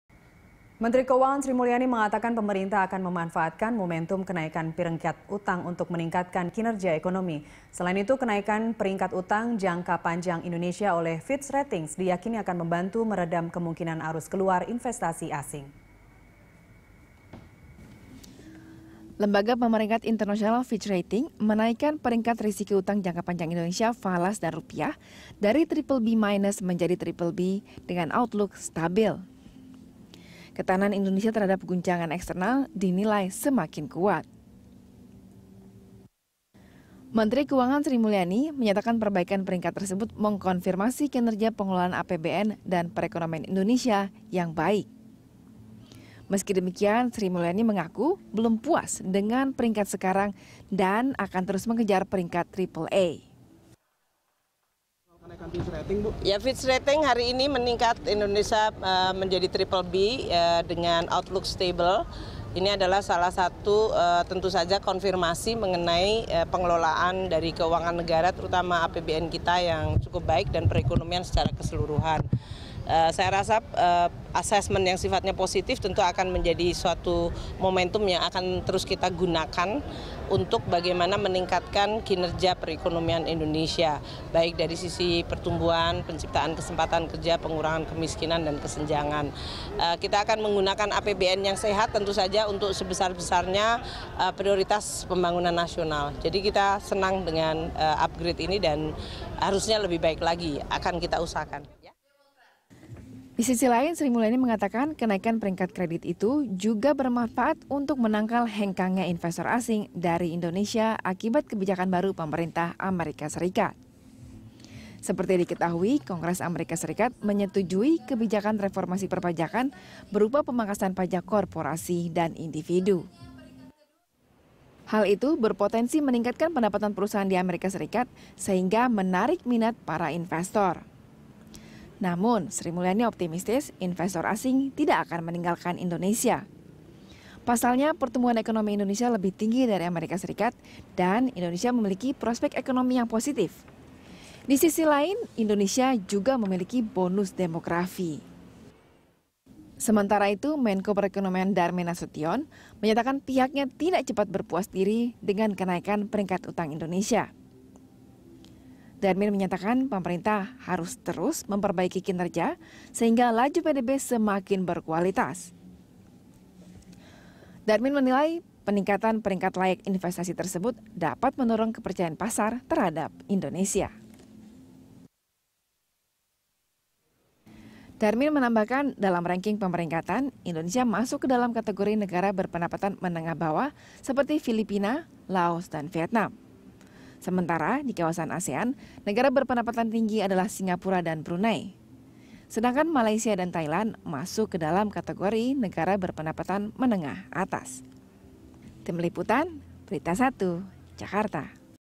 Menkeu: Peringkat Fitch Ratings Jadi Momentum Tingkatkan Kinerja Ekonomi
Menteri Keuangan Sri Mulyani mengatakan Pemerintah akan memanfaatkan momentum kenaikan peringkat utang untuk meningkatkan kinerja ekonomi. Kenaikan peringkat utang jangka panjang Indonesia oleh Fitch Ratings diyakini akan membantu meredam kemungkinan arus keluar investasi asing.

Facebook.com/BeritaSatuTV
Youtube.com/BeritaSatu